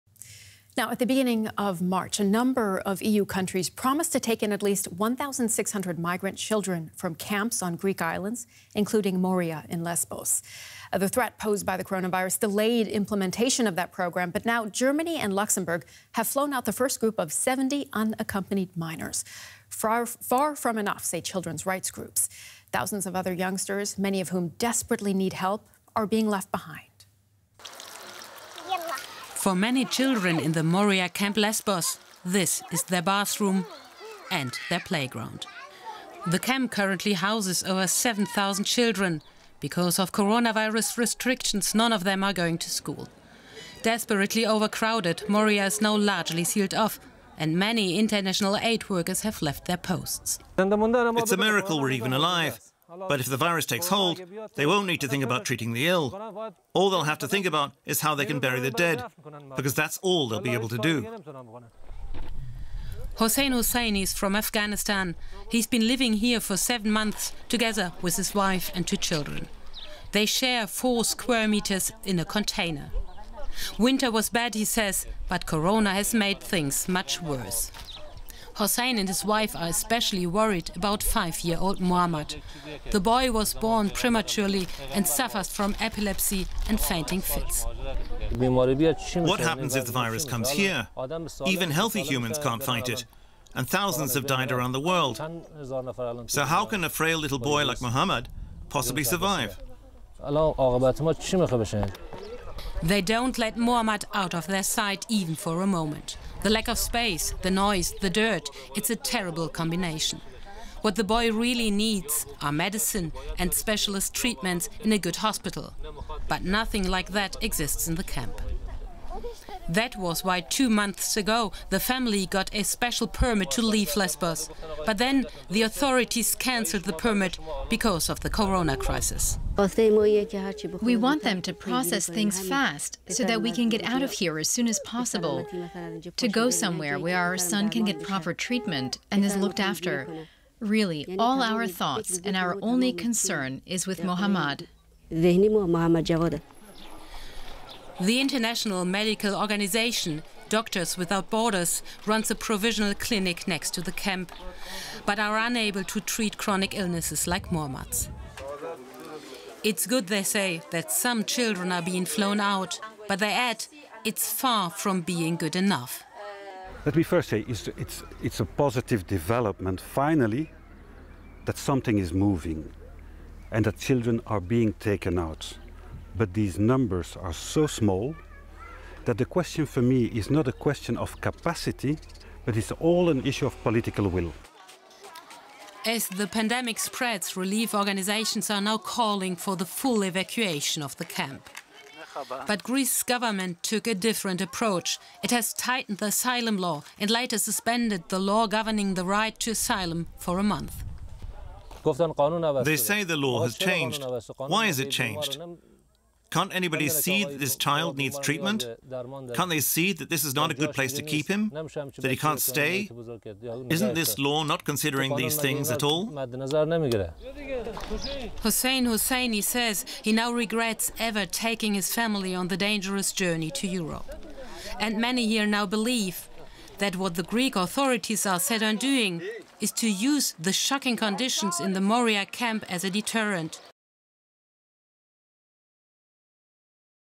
Coronavirus leaves refugee children stranded in Greek camps | DW News
In early March, a number of EU countries promised to take in at least 16 hundred migrant children from camps on Greek islands, including Moria on Lesbos. The coronavirus pandemic delayed implementation of the program, but now Germany and Luxembourg have flown out the first groups of 70 unaccompanied minors. Children’s rights organizations say that's nowhere near enough because thousands of other youngsters, many of whom desperately need help, are being left behind.